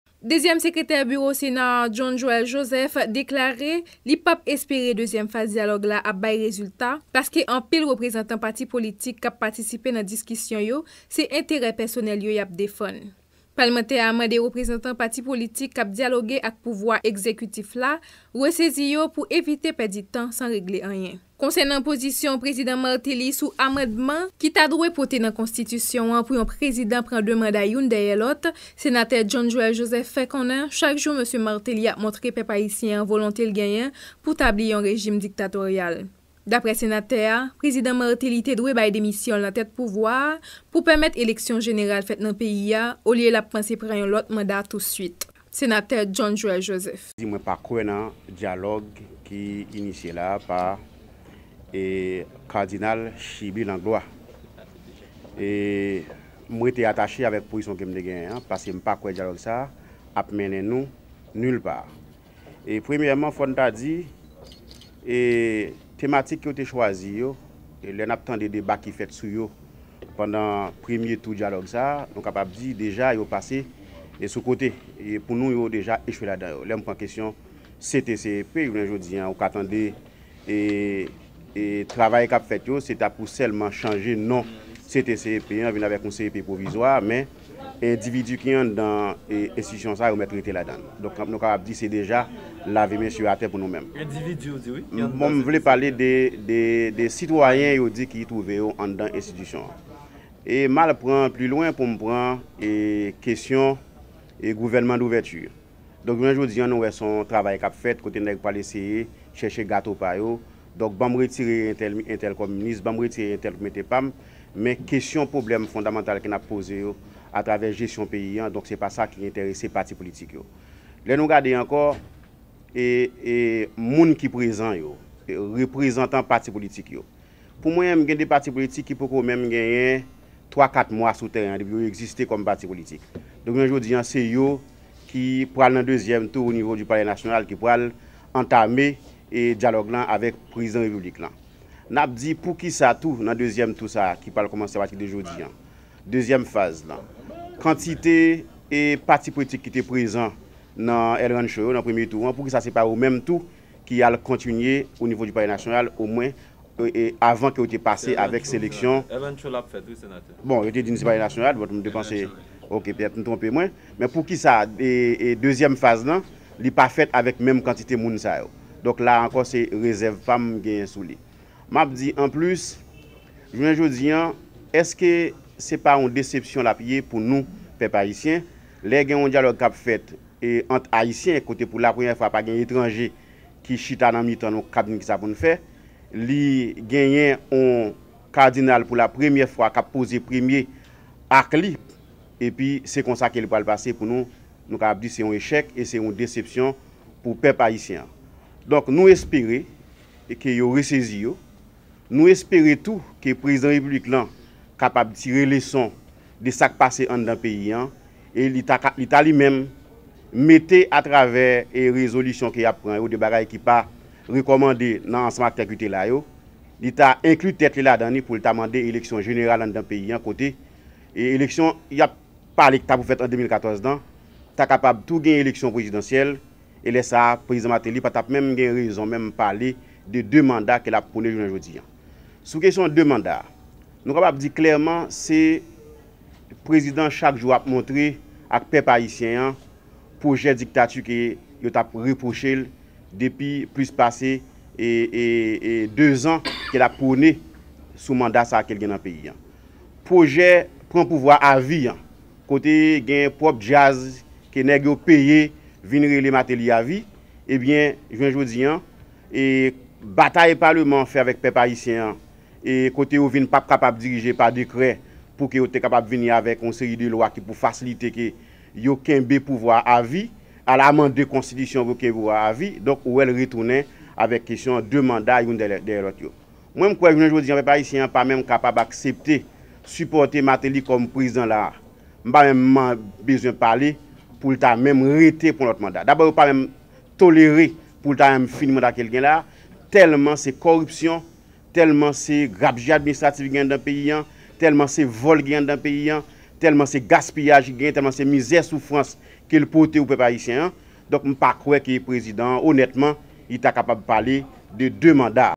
Dialogue National: Réactions du Sénateur de l'Ouest, John Joël Joseph
Au moment de mettre sous presse à 23h ce vendredi 14 février 2014, aucun accord n'avait encore été signé entre l'exécutif, le parlement et les partis politiques en raison principalement du retard accusé par l'exécutif dans la publication dans la gazette officielle, Le Moniteur, du nom des 10 membres de la Cour Supérieure des Comptes et du Contentieux Administratif tel qu'exigé par le Sénat de la République.

Le Cardinal Chibly Langlois qui s'est fait pilote de ces négociations avait pourtant promis au courant de la journée du vendredi 14 février 2014 que l'exécutif s'était engagé à publier les noms en ce jour d'hui. Toutefois, tel n'a pas été le cas jusqu'au moment de mettre sous presse.

Ce processus déjà fragilisé par le départ de plusieurs partis politiques d'importance risque de s'effriter davantage alors que le Cardinal Chibly Langlois et le Président Michel Martelly doivent s'envoler dans le prochaines heures pour l'Europe dans le cadre de l'élévation officielle du Monseigneur Langlois au rang de Cardinal.